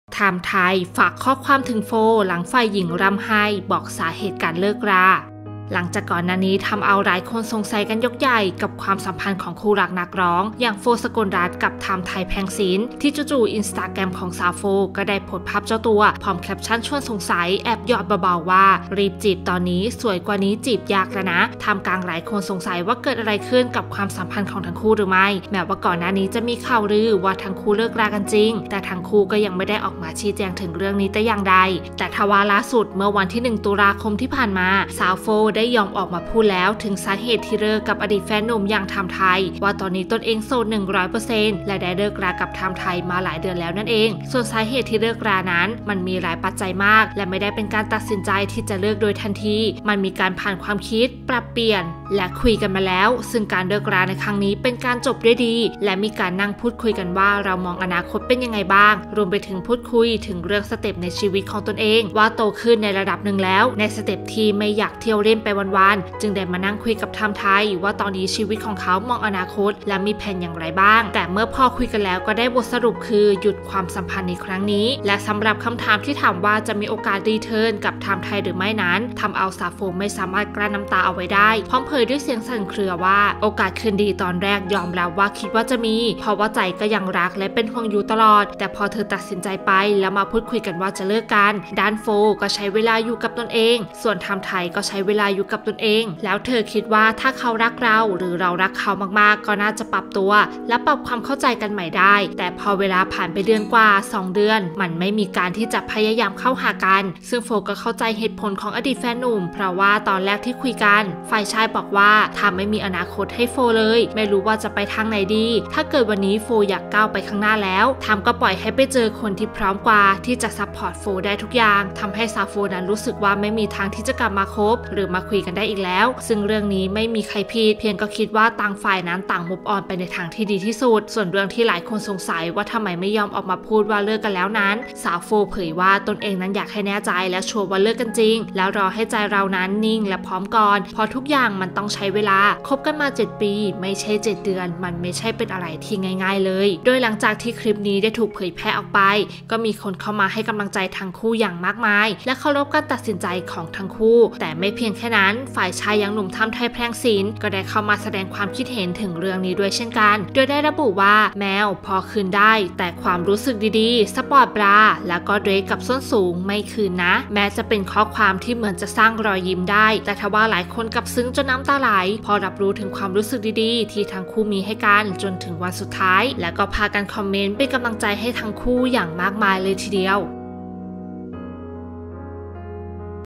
ธามไท ฝากข้อความถึง โฟร์ หลังฝ่ายหญิงร่ำไห้ บอกสาเหตุการเลิกรา
หลังจากก่อนหน้านี้ทำเอาหลายคนสงสัยกันยกใหญ่ กับความสัมพันธ์คู่รักนักร้อง โฟร์ ศกลรัตน์ กับ ธามไท เมื่อจู่ๆ อินสตาแกรมของสาวโฟร์ ได้โพสต์ภาพตัวเองพร้อมแคปชั่นชวนสงสัย หยอดเบาๆว่า รีบจีบตอนนี้ สวยกว่านี้จีบยากละนะ ท่ามกลางหลายคนสงสัยว่าเกิดอะไรกับความสัมพันธ์ของทั้งคู่หรือไม่ แม้ก่อนหน้านี้จะมีข่าวลือออกมาว่าทั้งคู่เลิกกันจริง แต่ทั้งคู่ก็ยังไม่ได้ออกมาชี้แจงแต่อย่างใด